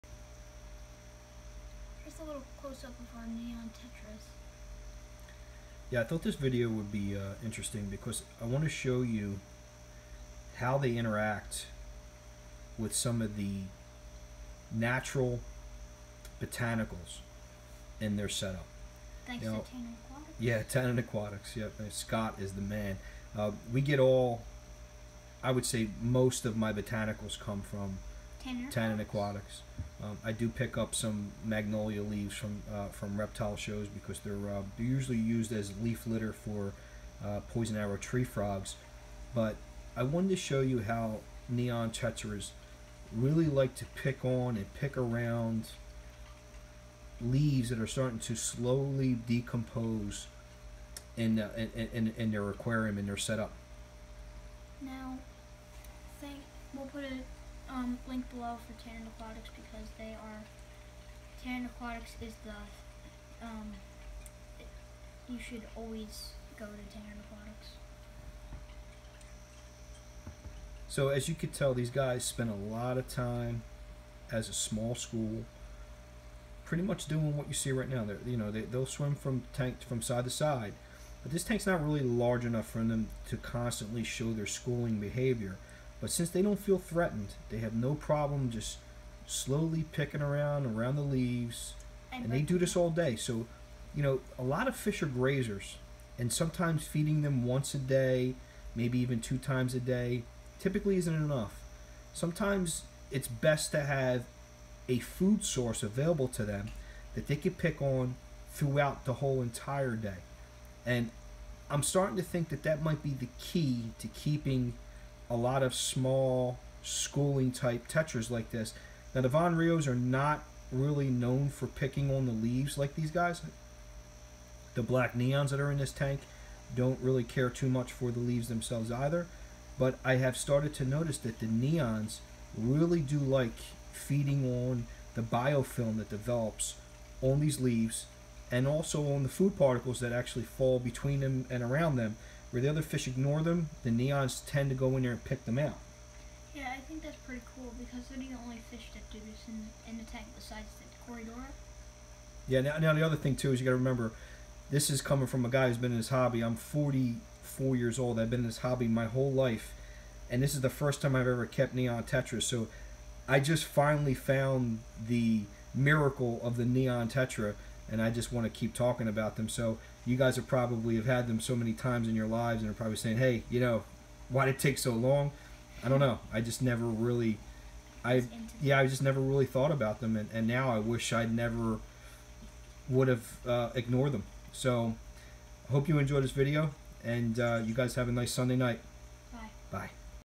Neon tetras
I finally found out why Neon tetras are on everybody's list of fish to have. I wanted to show these guys feeding and picking on the biofilm that grows on the magnolia leaves. These guys love to graze all day. Some of the neons I bought were tiny and I was concerned that they would be able to compete. They proved me wrong and have found their feeding Niche. Hope you enjoy!